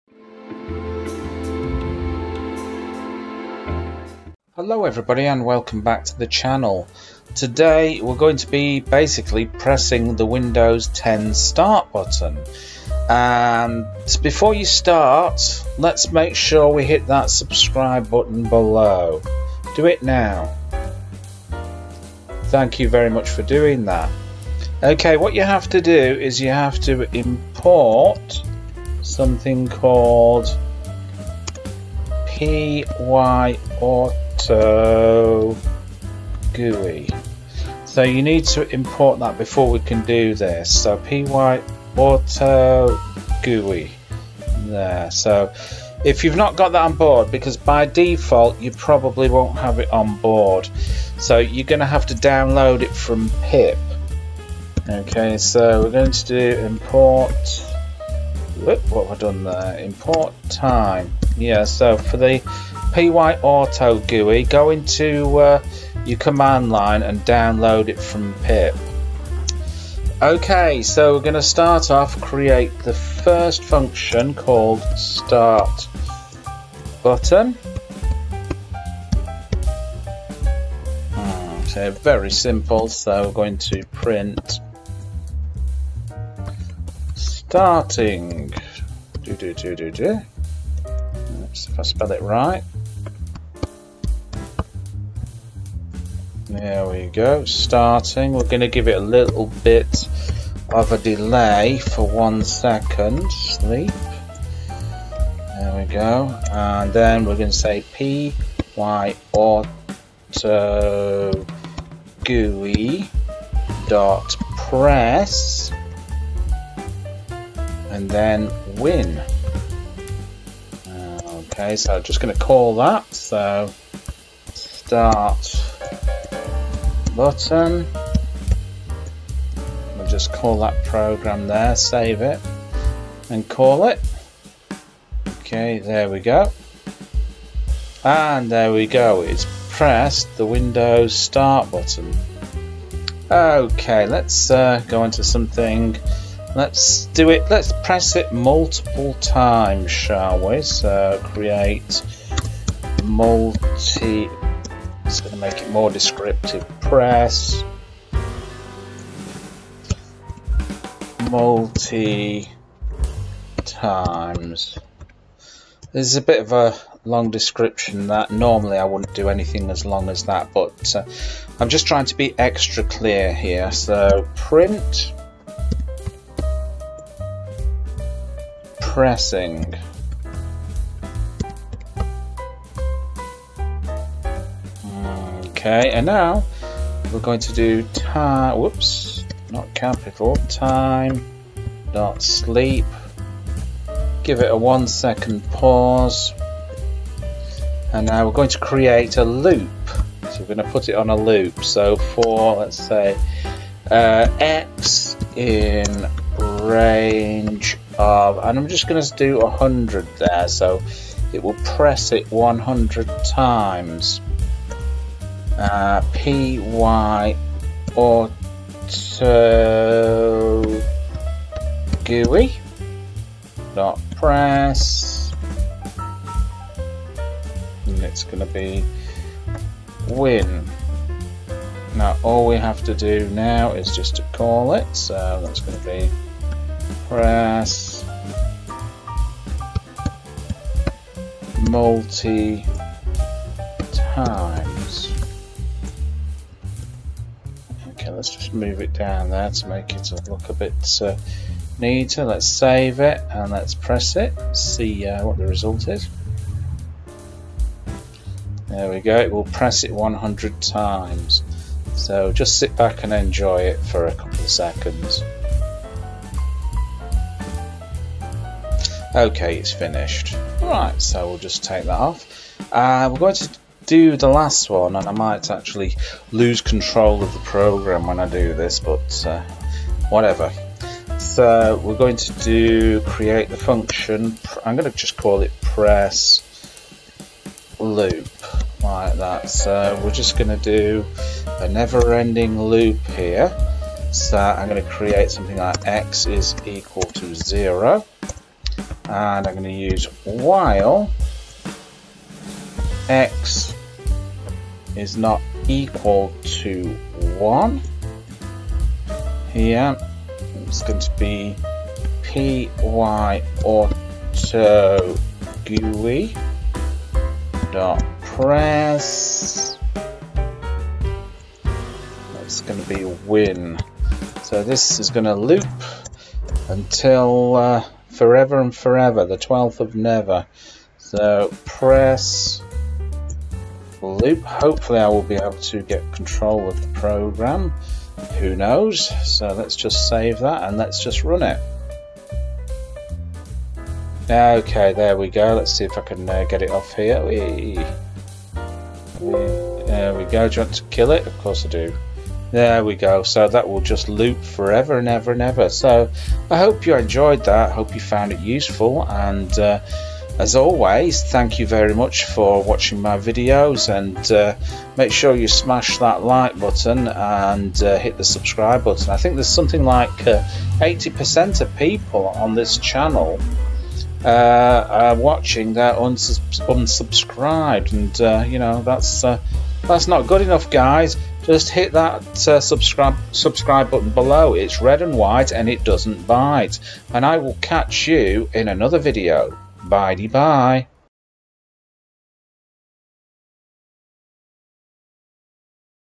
Python PyAutoGui Windows Start Infinite Loop 2023
Python Code Using Pyautogui To Press The Windows 10 Start Menu. Easy Fun Code For Beginners Making Use Of The Infinite Loop.